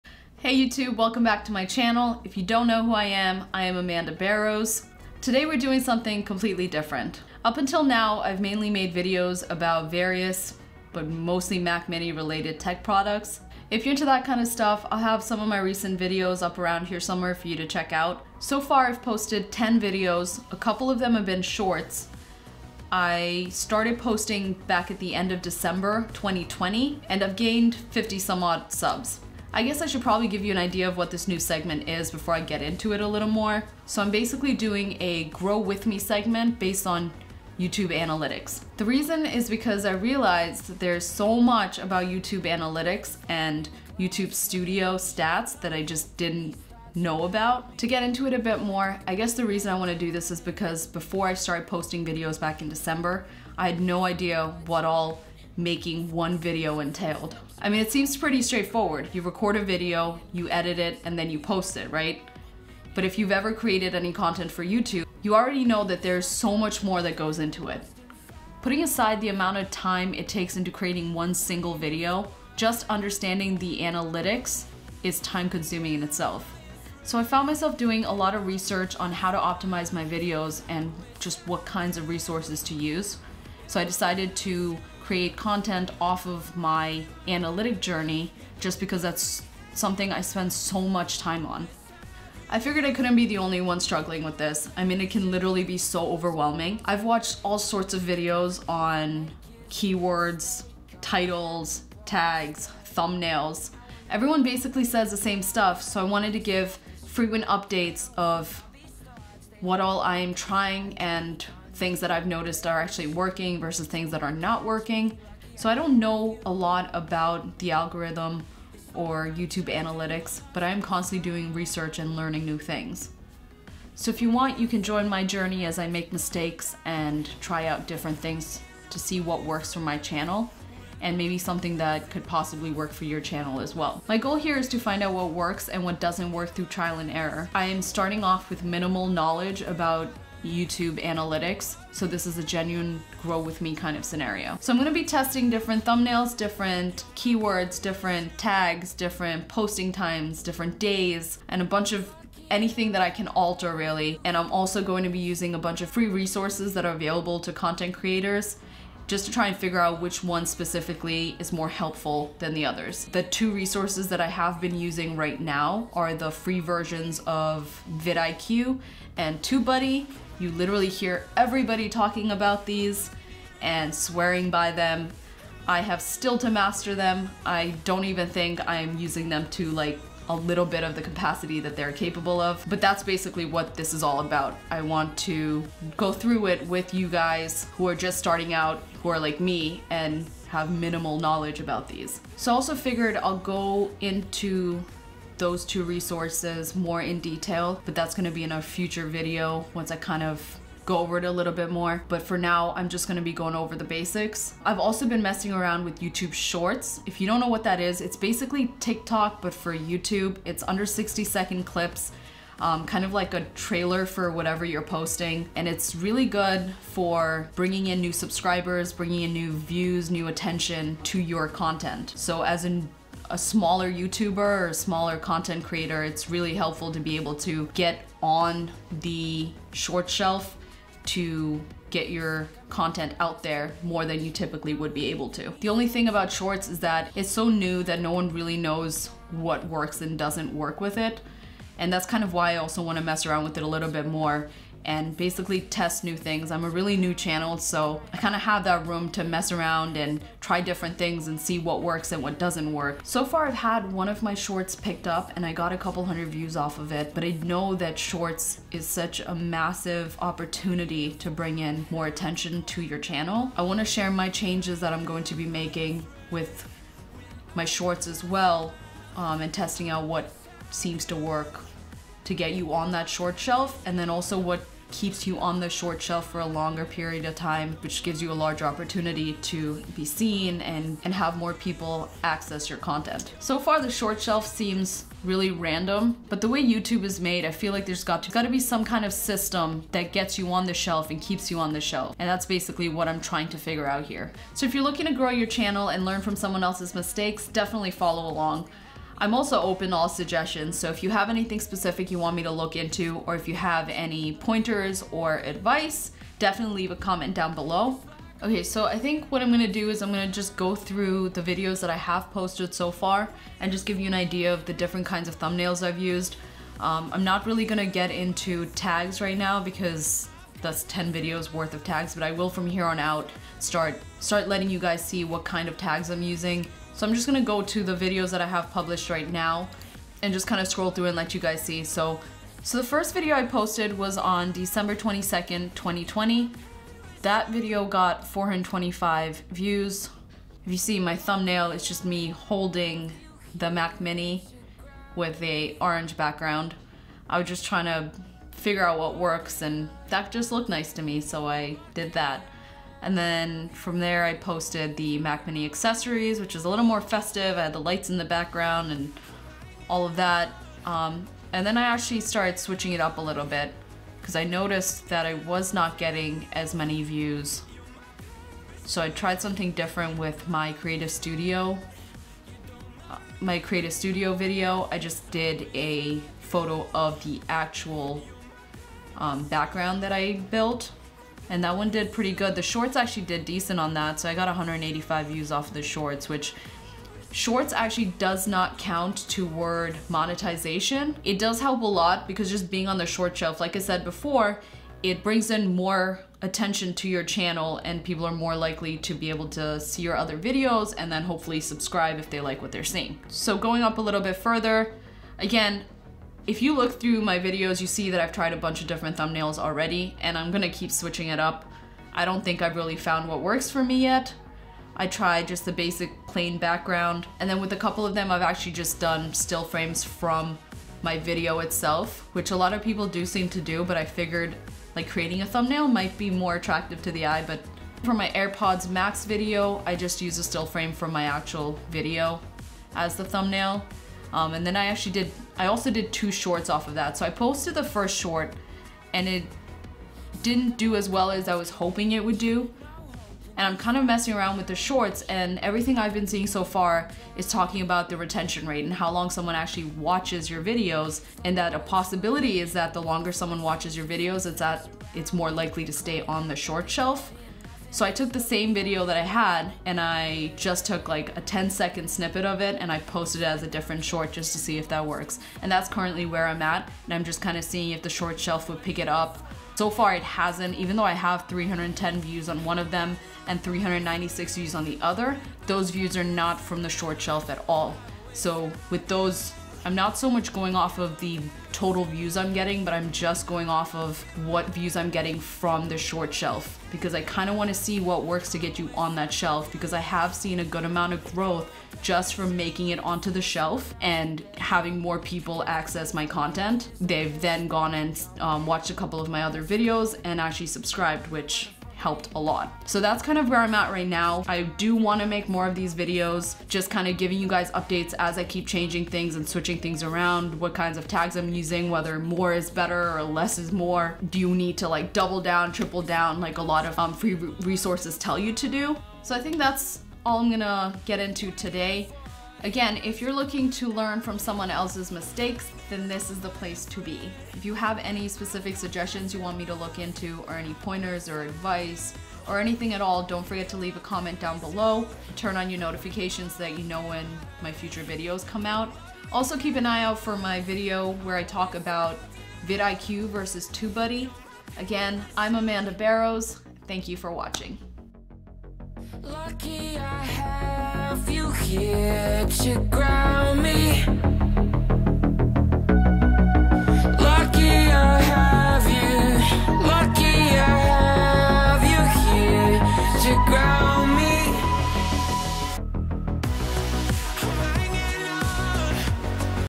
Youtube Analytics Intro | How to understand the Algorithm/ Video Optimization | TubeBuddy vs VIDIQ
I spend so much time trying to figure out Youtube analytics and the Algorithm, and trying to understand what's the best way to optimize my Tech Videos. This is an introduction to my new segment where i'll be tracking my progress and failures and detailing everything I try out and what the results are. Hopefully this helps someone else who, like me, is just starting out and a bit overwhelmed by all the information! I'll also be going over different free/paid resources available to content creators, like TubeBuddy and VIDIQ!